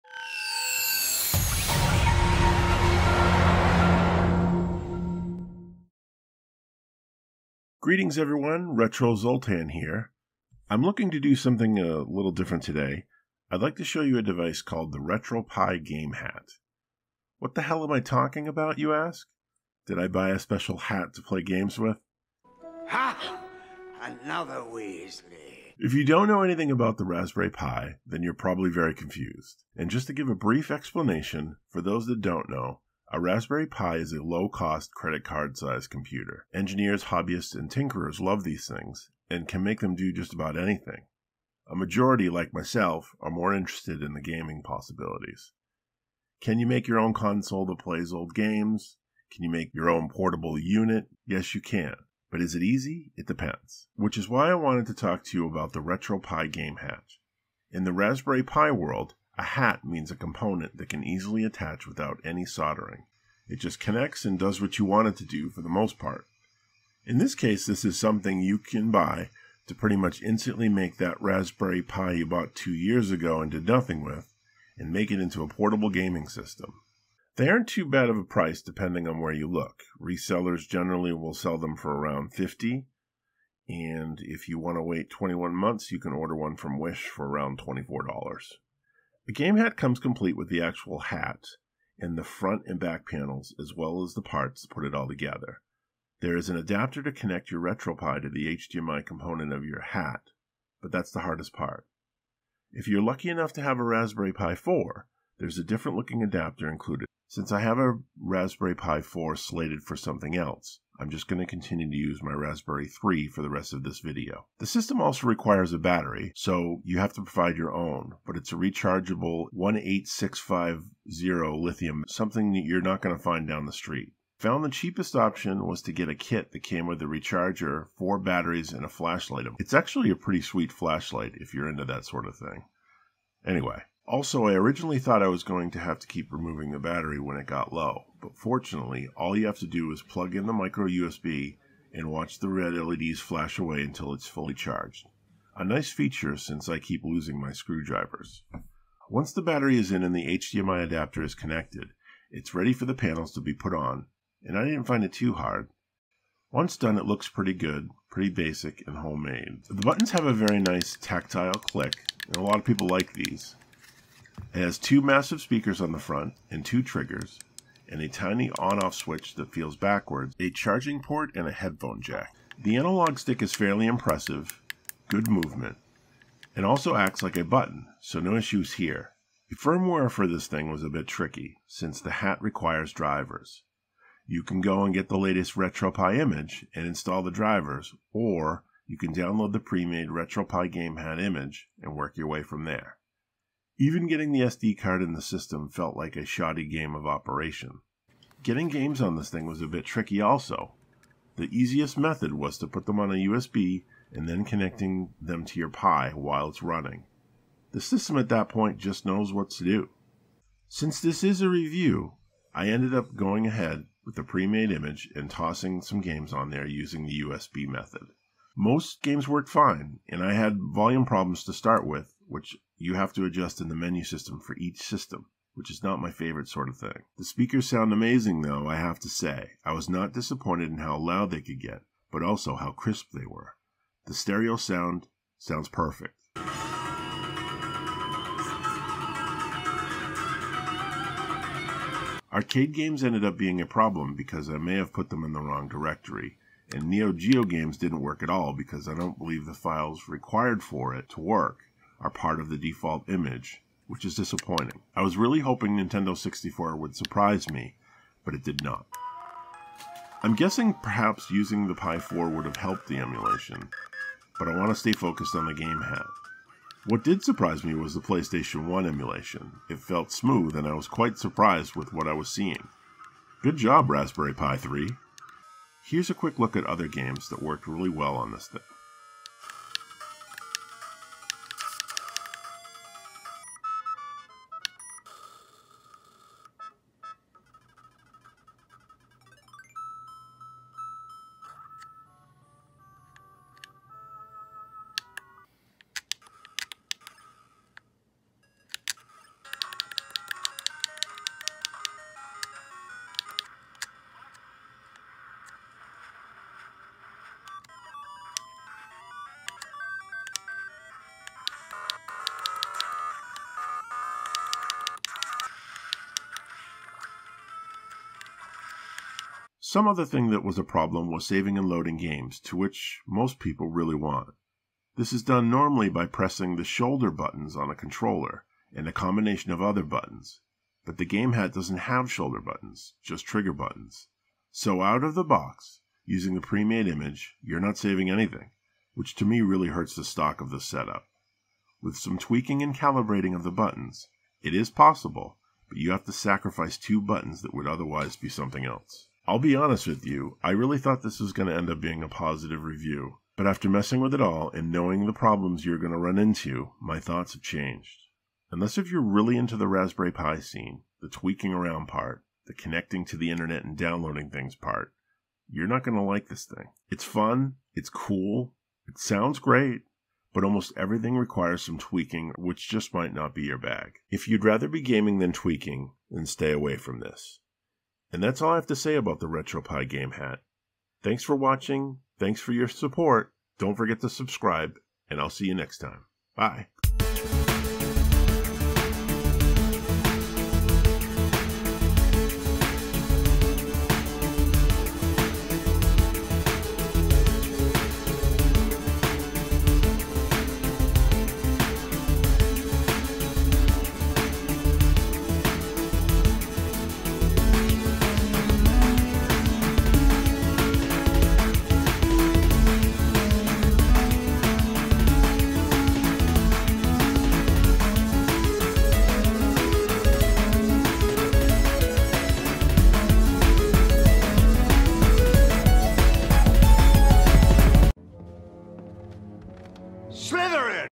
WaveShare Game Hat Review Raspberry Pi Handheld Console
I have wanted to do this forever, but now that I did I'm kind of not sure what to think. Have you tried this, and if so what do you think? Would you want to try this after what you have seen in this video? I would be curious to know!

The WaveShare Raspberry Pi Hat

The flashlight link (in case if anyone asks)

Zoltan Booth Artwork : Nuvvola
Music (Intro and Credits): Lance Bembry
Background Animation: from Pexels

Special Thanks to all my supporters (if your reading this, you are one of them!)